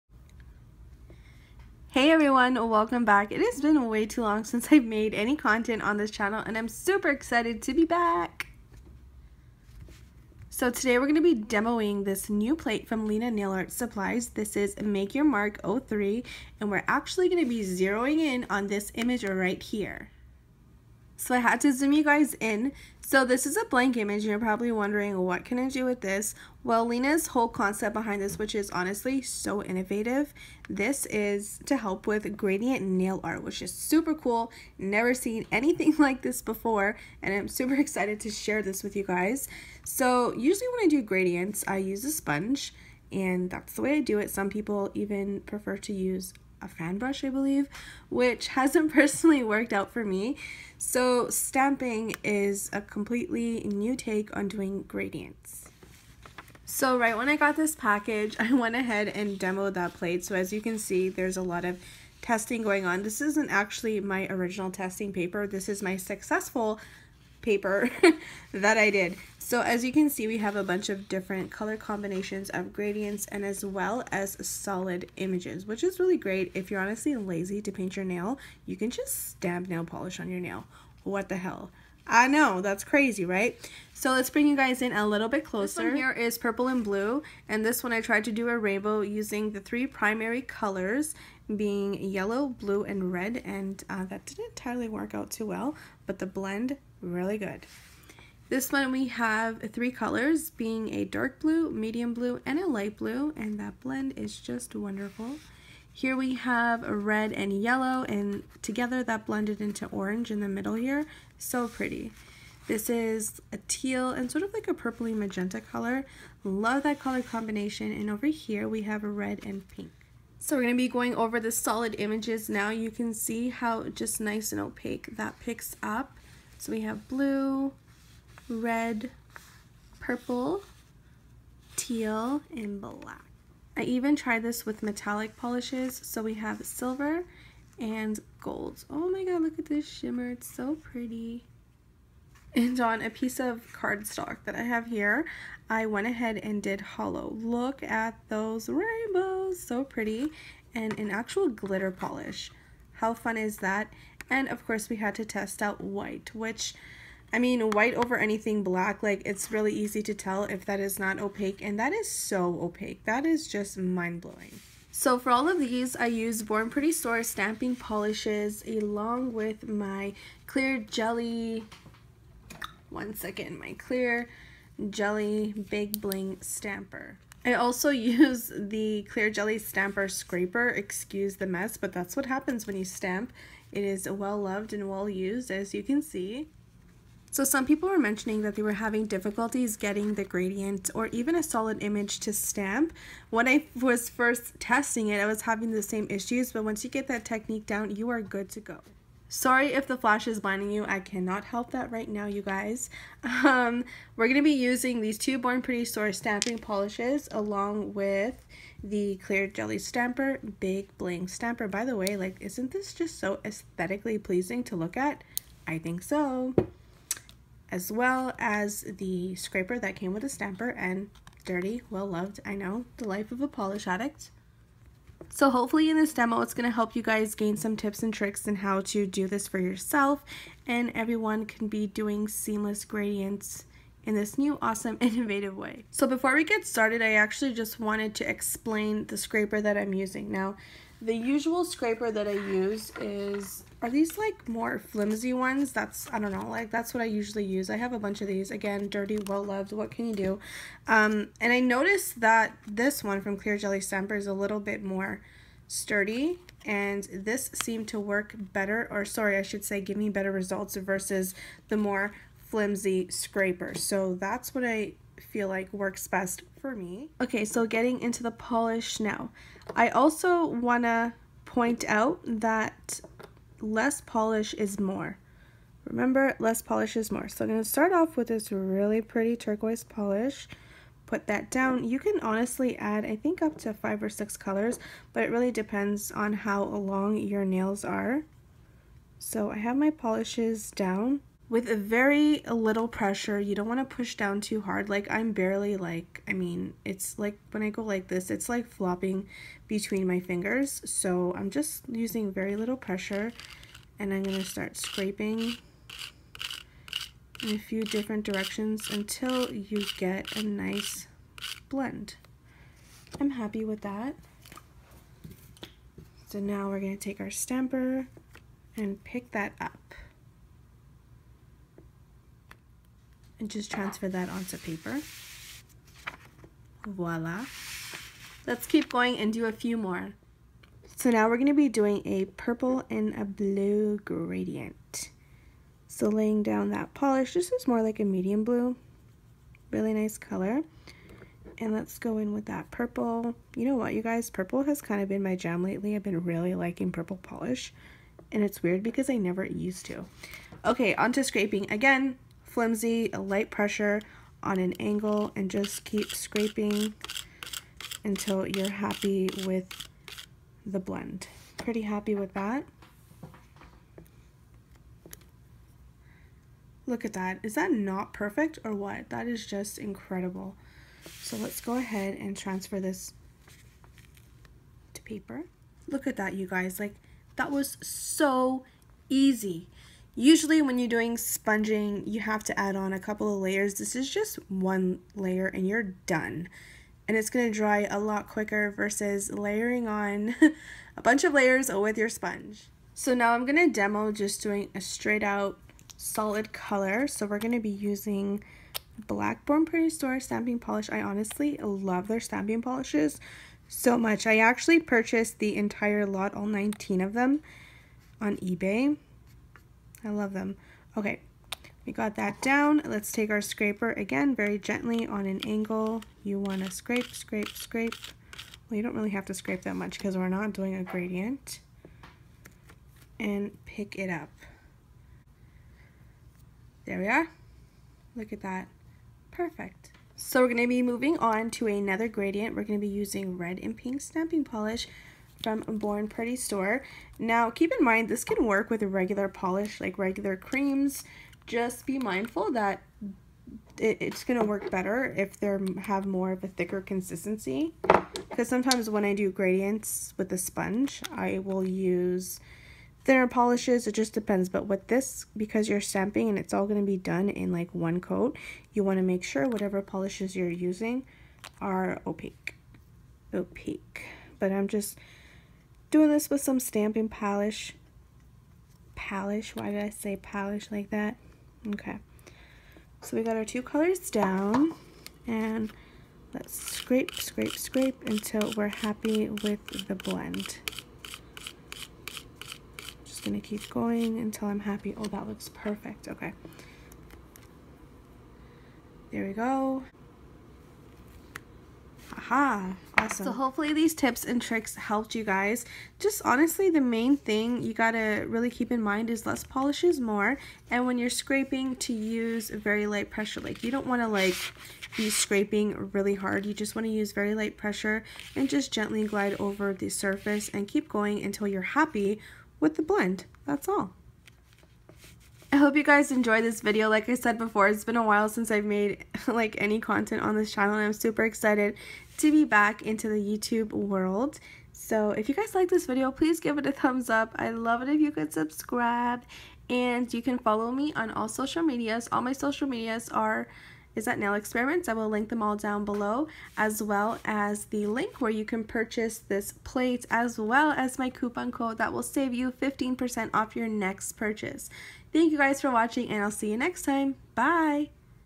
REVIEW | LINA NAIL ART SUPPLIES MAKE YOUR MARK 03 BLANK IMAGE TIPS
Who knew you could do GRADIENT nails with nail stamping?!

This blank image is incredibly innovative for fellow nail stamping lovers and hope my tips & tricks relieve any questions you may have about this specific image.